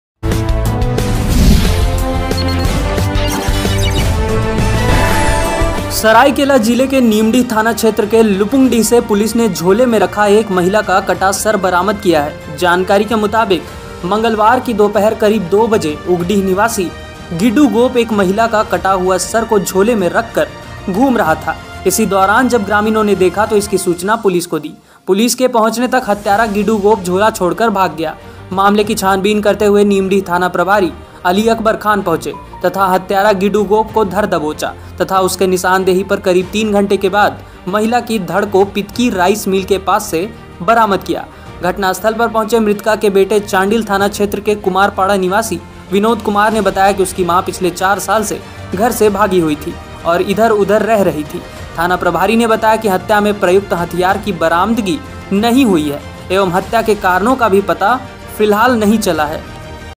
झोले में कटा सर लेकर घूम रहा व्यक्ति हुआ गिरफ्तार, पुलिस जांच में जुटी
जानकारी के मुताबिक मंगलवार की दोपहर करीब 2 बजे उगडीह निवासी गिडू गोप एक महिला की कटा सिर को झोले में रखकर घूम रहा था। इसी दौरान जब ग्रामीणों ने देखा तो इसकी सूचना पुलिस को दिया। पुलिस के पहुंचने तक हत्यारा गिडू गोप झोला छोड़कर भाग गया।
Breaking News Dhanbad Jharkhand Bihar